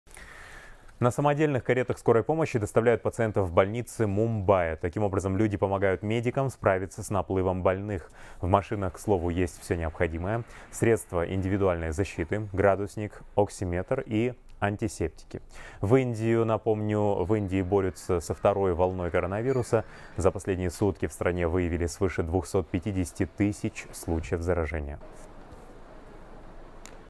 Самодельные кареты скорой помощи пустили в ход в Индии
На самодельных каретах скорой помощи доставляют пациентов в больницы Мумбая, сообщает корреспондент «Хабар 24».

Таким образом люди помогают медикам справиться с наплывом больных. В машинах, к слову, есть всё необходимое: средства индивидуальной защиты, градусник, оксиметр и антисептики.

Мумбаи готовится к третьей волне COVID-19

В Индии, напомним, борются со второй волной коронавируса. Только за последние сутки в стране выявили свыше 250 тысяч случаев.